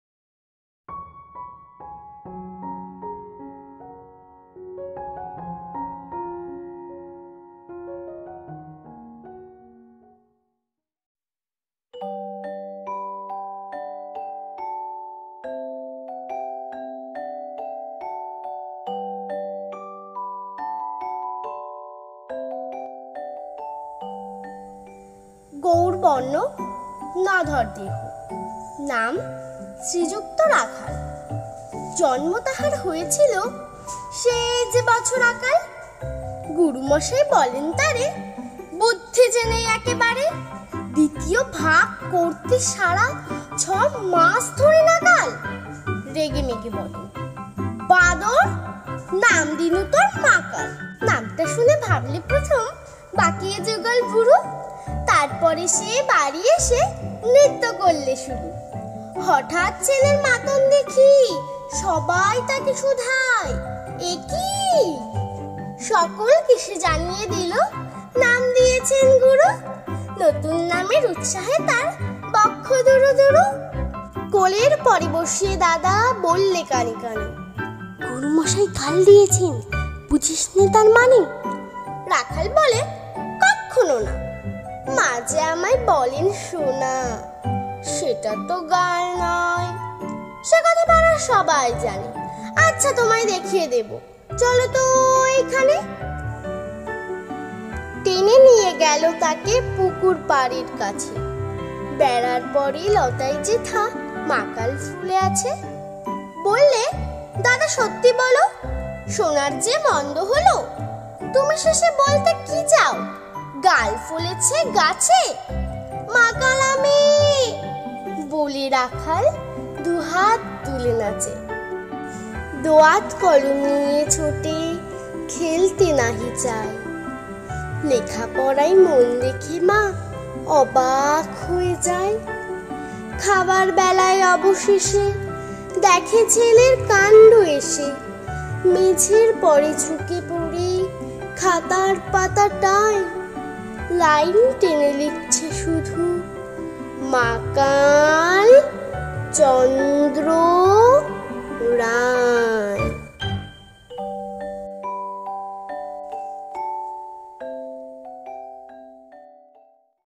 Makal | Rabindranath Thakur | Recited by Titan | মাকাল | রবীন্দ্রনাথ ঠাকুর | আবৃত্তিতে তিতান
মাকাল
- রবীন্দ্রনাথ ঠাকুর

গৌরবর্ণ নধর দেহ, নাম শ্রীযুক্ত
রাখাল,
জন্ম তাহার হয়েছিল, সেই যে-বছর
আকাল।
গুরুমশায় বলেন তারে,
"বুদ্ধি যে নেই একেবারে;
দ্বিতীয়ভাগ করতে সারা ছ'মাস
ধরে নাকাল।"
রেগেমেগে বলেন, "বাঁদর, নাম
দিনু তোর মাকাল।"
নামটা শুনে ভাবলে প্রথম
বাঁকিয়ে যুগল ভুরু;
তারপর সে বাড়ি এসে নৃত্য করলে
শুরু।
হঠাৎ ছেলের মাতন দেখি
সবাই তাকে শুধায়, এ কী!
সকলকে সে জানিয়ে দিল, নাম
দিয়েছেন গুরু--
নতুন নামের উৎসাহে তার বক্ষ দুরুদুরু।
কোলের 'পরে বসিয়ে দাদা বললে
কানে-কানে,
  "গুরুমশায় গাল দিয়েছেন, বুঝিসনে
তার মানে!"
রাখাল বলে, "কখ্খোনো না,
মা যে আমায় বলেন সোনা,
সেটা তো গাল নয় সে কথা
পাড়ার সবাই জানে।
আচ্ছা, তোমায় দেখিয়ে দেব,
চলো তো ঐখানে।"
টেনে নিয়ে গেল তাকে
পুকুরপাড়ের কাছে,
বেড়ার 'পরে লতায় যেথা মাকাল
ফ'লে আছে।
বললে, "দাদা সত্যি বোলো,
সোনার চেয়ে মন্দ হল?
তুমি শেষে বলতে কি চাও, গাল
ফলেছে গাছে।"
"মাকাল আমি" ব'লে রাখাল দু হাত
তুলে নাচে।
দোয়াত কলম নিয়ে ছোটে,
খেলতে নাহি চায়,
লেখাপড়ায় মন দেখে মা অবাক
হয়ে যায়।
খাবার বেলায় অবশেষে
দেখে ছেলের কাণ্ড এসে--
মেঝের 'পরে ঝুঁকে প'ড়ে খাতার
পাতাটায়
  লাইন টেনে লিখছে শুধু--
মাকালচন্দ্র রায়।

‘গুরুমশায় বলেন তারে/‌বুদ্ধি যে নেই একেবারে/দ্বিতীয়ভাগ করতে সারা ছ’মাস ধরে নাকাল।’ রেগেমেগে বলেন, ‘বাঁদর, নাম দিনু তোর মাকাল।’ রবীন্দ্রনাথ ঠাকুর এভাবেই শিশুতোষ ‘মাকাল’ নামক কবিতায় অনুন্নত বালকটি পরিচয় তুলে ধরেছিলেন।

এ নামটি ধীরে ধীরে হারিয়ে গিয়ে উপমাশ্রিত নামে রূপান্তরিত হয়েছে। এর বৈজ্ঞানিক নাম Trichosanthes tricuspidata।
টকটকা লাল ফল মাকাল। উদ্ভিদটি ওষুধি গুণাগুণসম্পন্ন হলেও বিপন্ন প্রকৃতিতে তার অস্তিত্ব আরও সংকটে। প্রকৃতি থেকে হারিয়ে যেতে বসেছে এ লতানো উদ্ভিদটি।

সৃষ্টিকর্তা পৃথিবীতে কিছু উপমা তৈরি করেছেন, মানুষকে সচেতন করার জন্য। মাকাল ফল হলো সেই উপমা। অর্থাৎ চকচক করলেই সোনা হয় না। শুধু চককের দিকে তাকালে হবে না, এর কোয়ালিটির দিক আগে দেখতে হবে। এই উপমার জন্যই মাকাল নামকরণ। মাকাল ফলের বাইরের দিক সুন্দর হলেও ভেতরটা দেখতে অসুন্দর।

এটি ওষুধি গাছ। মাকাল এখন বাংলাদেশে খুবই কম; নেই বললেই চলে। এ ফুলের আদি নিবাস তুর্কি। আমাদের দেশে একসময় প্রচুর ছিল। এ উদ্ভিদটি লতা জাতীয় হলেও এরা বড় গাছে আশ্রয় করে বেঁচে থাকে। যেহেতু বড়গাছ প্রায় নেই বললেই চলে, তাই তাদের সংখ্যাও কমে গেছে। এ লতানো উদ্ভিদটি সব সময় হাতের নাগালের বাইরে থাকে।

ফলের স্বাদ ও তুলনামূলক পার্থক্য উল্লেখ করে ড. মোহাম্মদ জসীম উদ্দিন বলেন, ফলটি তেতো জাতীয়। ফলের ভেতরের রংটার সঙ্গে উপরের রঙের কোনো প্রকার সামঞ্জস্যতা নেই। ভেতরের পচা রঙের সঙ্গে উপরের সুন্দর লাল টকটকা রঙের কোনো মিল নেই। বাইরের রং এতো সুন্দর যে, মানুষ এটা দেখে আকৃষ্ট হয়।

মাকাল ফলটি আমাদের সমাজের চমৎকার উদাহরণ। সমাজে তো আমাদের অনেক সুন্দর মানুষ আছেন, শুধু তাদের সৌন্দর্য দেখলেই হবে না, তাদের গুণগুলোও যাচাই করা প্রয়োজন।

Credits :
Video : Soumyadeep Bhunia